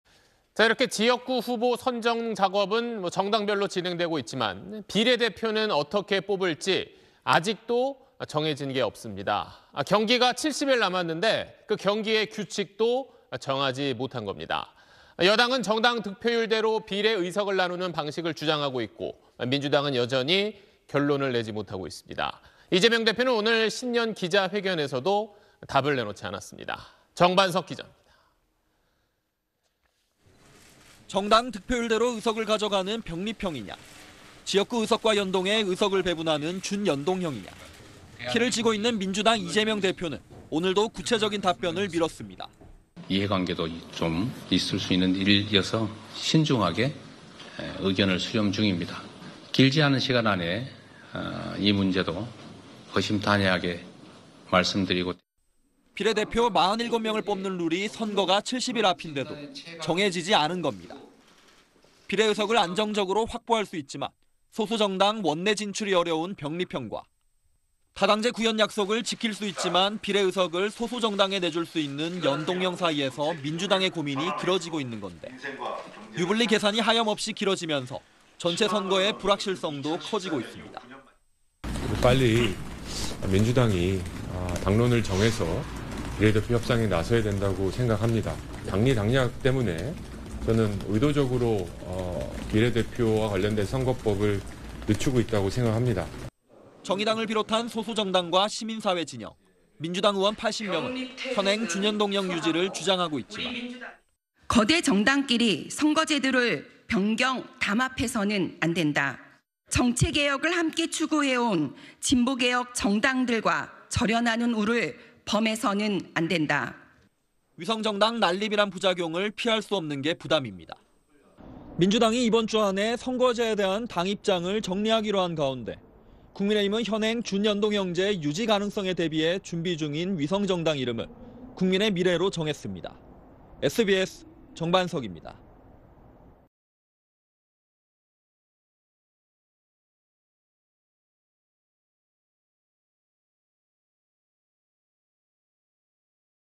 총선 70일 앞인데 '미정'…'비례 대표' 두고 유불리 계산만 / SBS 8뉴스
〈앵커〉

이렇게 지역구 후보 선정 작업은 정당별로 진행되고 있지만, 비례대표는 어떻게 뽑을지, 아직도 정해진 게 없습니다. 경기가 70일 남았는데, 그 경기의 규칙도 정하지 못한 겁니다. 여당은 정당 득표율대로 비례 의석을 나누는 방식을 주장하고 있고 민주당은 여전히 결론을 내지 못하고 있습니다. 이재명 대표는 오늘(31일) 신년 기자회견에서도 답을 내놓지 않았습니다.

정반석 기자입니다.

〈기자〉

정당 득표율대로 의석을 가져가는 병립형이냐, 지역구 의석과 연동해 의석을 배분하는 준연동형이냐.

키를 쥐고 있는 민주당 이재명 대표는 오늘도 구체적인 답변을 미뤘습니다.

[이재명/민주당 대표 : 이해관계도 좀 있을 수 있는 일이어서 신중하게 의견을 수렴 중입니다. 길지 않은 시간 안에 이 문제도 허심탄회하게 말씀드리고 (대화할 시간이 있을 겁니다.)]

비례대표 47명을 뽑는 룰이 선거가 70일 앞인데도 정해지지 않은 겁니다.

비례 의석을 안정적으로 확보할 수 있지만, 소수 정당 원내 진출이 어려운 병립형과 다당제 구현 약속을 지킬 수 있지만, 비례 의석을 소수 정당에 내줄 수 있는 연동형 사이에서 민주당의 고민이 길어지고 있는 건데, 유불리 계산이 하염없이 길어지면서 전체 선거의 불확실성도 커지고 있습니다.

[조정훈/국민의힘 의원 : 빨리 민주당이 당론을 정해서 비례대표 협상에 나서야 한다고 생각합니다. 당리당략 때문에 의도적으로 비례대표와 관련된 선거법을 늦추고 있다고 생각합니다.]

정의당을 비롯한 소수정당과 시민사회진영, 민주당 의원 80명은 현행 준연동형 유지를 주장하고 있지만,

[한미경/전국여성연대 대표 : 거대 정당끼리 선거제도를 변경, 담합해서는 안 된다. 정치개혁을 함께 추구해 온 진보개혁정당들과 절연하는 우를 범해서는 안 된다.]

위성정당 난립이라는 부작용을 피할 수 없는 게 부담입니다.

민주당이 이번 주 안에 선거제에 대한 당 입장을 정리하기로 한 가운데, 국민의힘은 현행 준연동형제 유지 가능성에 대비해 준비 중인 위성정당 이름을 '국민의 미래'로 정했습니다.

(영상취재 : 설민환, 영상편집 : 이승열)